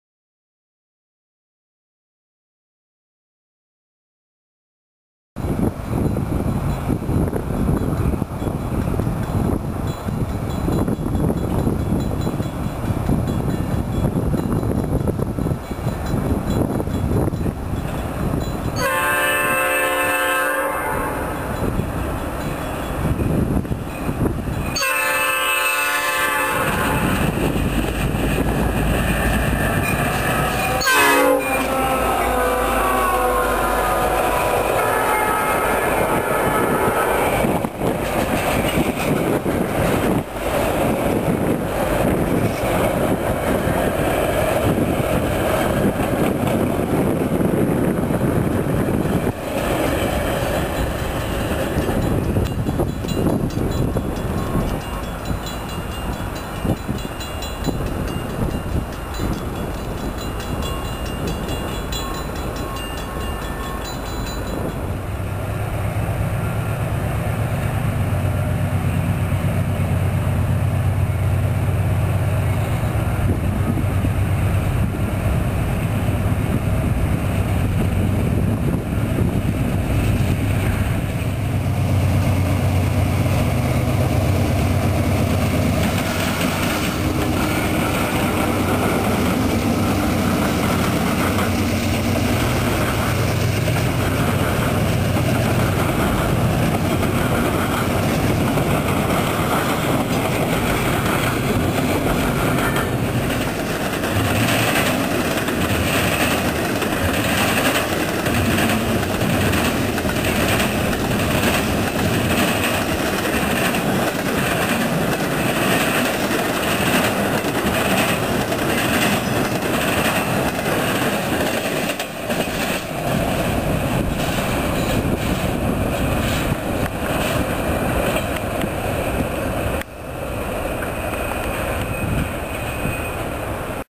Chicago Ridge Crossing: CSX and Metra/Southwest Service
Metra/SouthWest Service outbound train #815 to Manhattan, IL crosses the Indiana Harbor Belt/CSX diamonds at speed with two F40PHM-2s leading nine cars, many on loan to the line from the BNSF Line.  Train 815 crosses Ridgeland Ave and makes its station stop at Chicago Ridge.  Metra's Southwest Service operates over NS's ex-Wabash main line here.  Next, a CSX stack train accelerates through the crossing, with AC44009Ws #494 and 693 leading.